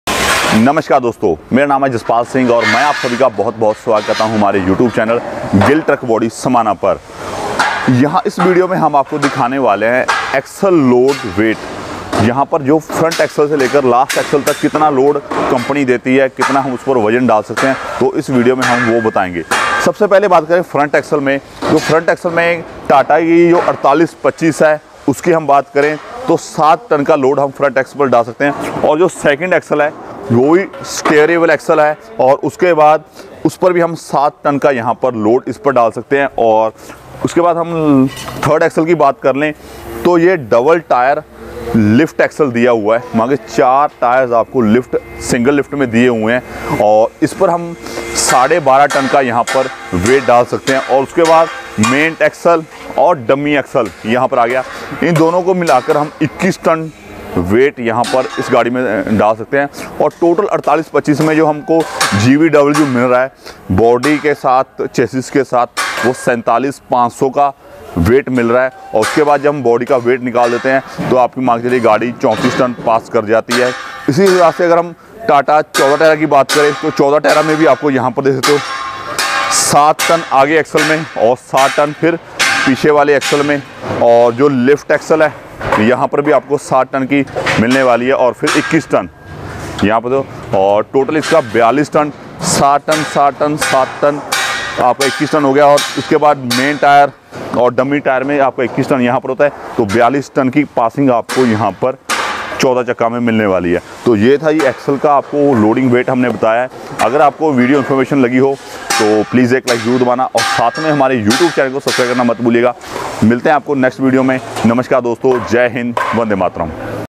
Truck Axles पे कितना WEIGHT डाल सकते है
IN THIS VIDEO YOU WILL AXLE LOAD CONFIGIRATION OF 16 WHEELER TRUCK AND 14 WHEELER TRUCK
TRUCKS PHOTOS - ‎Follow this link to view our catalog on WhatsApp

GILL Truck Body SAMANA 🤙🏼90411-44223